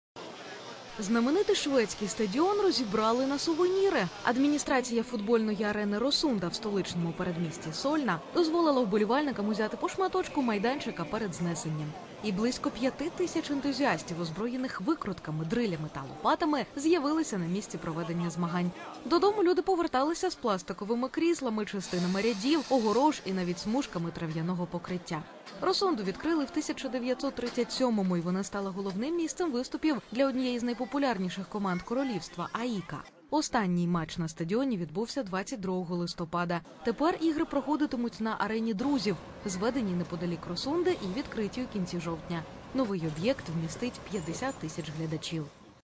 Знаменитий шведський стадіон розібрали на сувеніри
ubr.ua - украинский бизнес ресурс : Знаменитий шведський стадіон розібрали на сувеніри. Адміністрація футбольної арени "Росунда" в столичному передмісті Сольна дозволила вболівальникам узяти по шматочку майданчика перед знесенням. І близько 5-ти тисяч ентузіастів, озброєних викрутками, дрИлями та лопатами з'явилися на місці проведення змагань. Додому люди поверталися з пластиковими кріслами, частинами рядів, огорож і навіть смужками трав'яного покриття. "Росунду" відкрили в 1937-му, і вона стала головним місцем виступів для однієї з найпопулярніших команд королівства - "АІКи". Останній матч на стадіоні відбувся 22 листопАда. Тепер ігри проходитимуть на "Арені Друзів", зведеній неподалік&nbsp; "Росунди" і відкритій у кінці жовтня. Новий об"єкт вмістить 50 тисяч глядачів. .